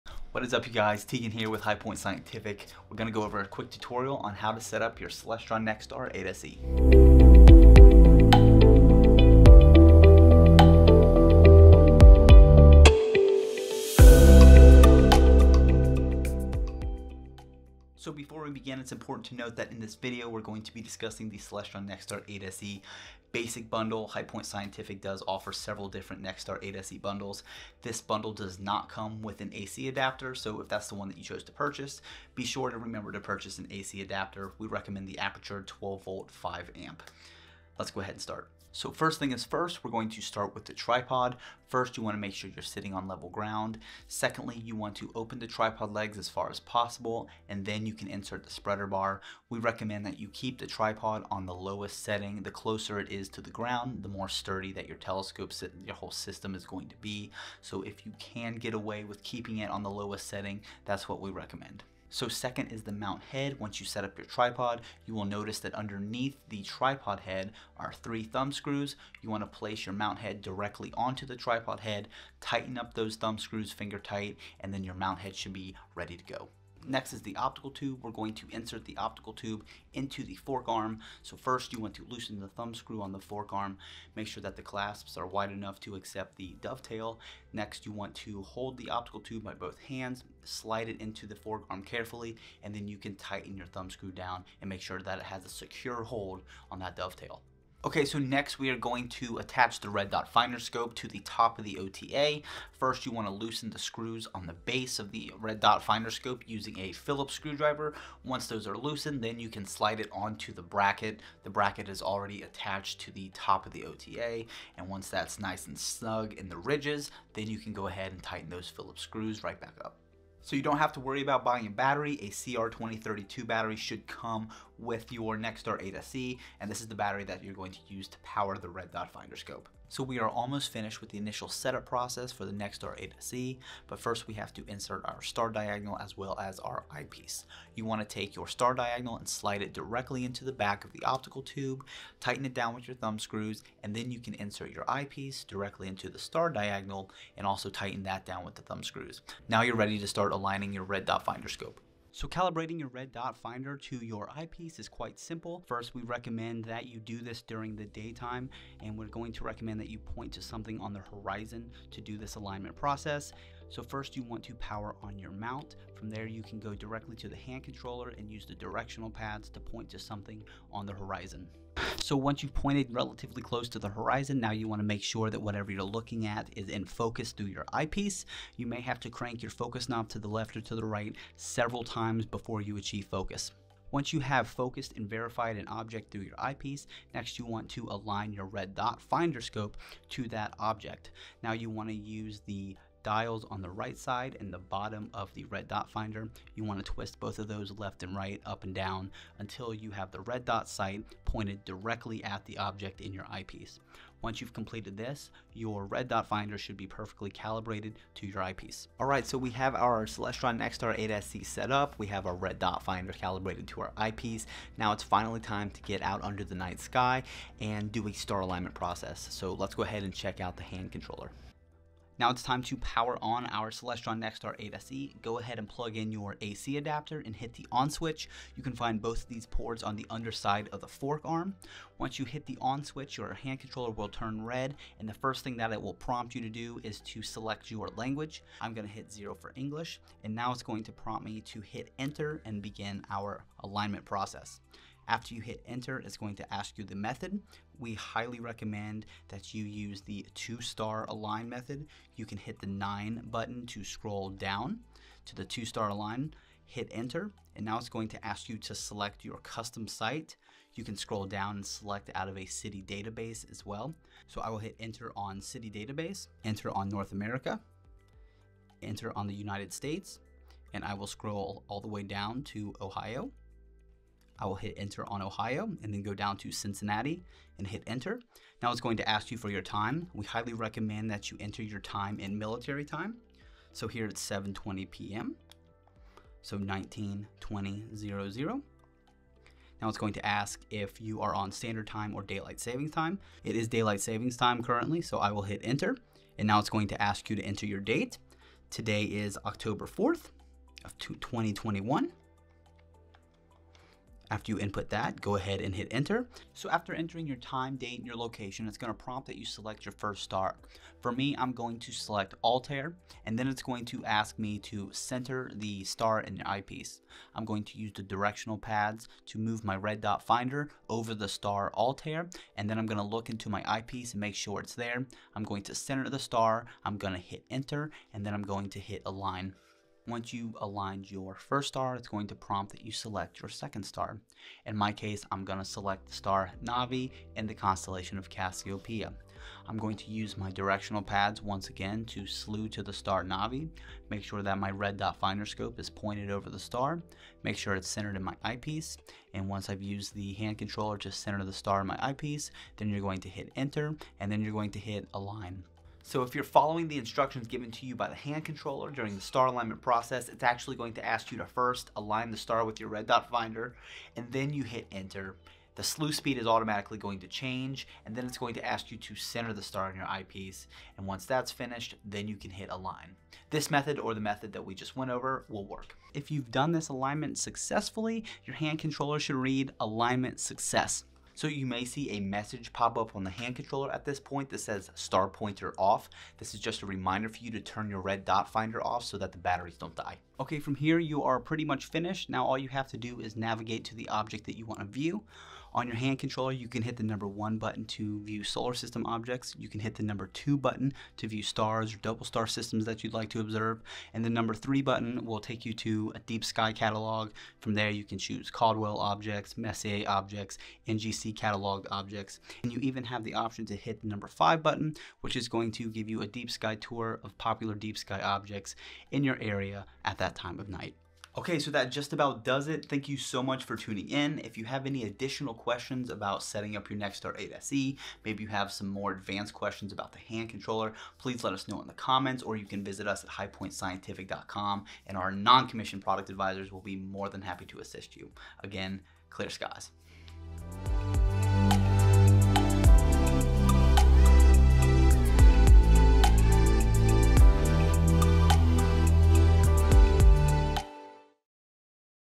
Setting Up a Celestron NexStar 8SE | High Point Scientific
In this video Teagan sets up a Celestron NexStar 8SE telescope.

The Celestron NexStar 8SE is a fantastic telescope that can show incredible detail on the Moon, Planets, double stars, nebula, and so much more. The telescope features an 8" (203mm) primary mirror and a focal length of 2032mm f/10. The telescope comes with the Celestron NexStar SE GoTo altazimuth mount, which can be setup and aligned in a manner of minutes. Additionally, the NexStar SE mount is GPS compatible, and with the hand controller has a database of nearly 40,000 objects in the night sky.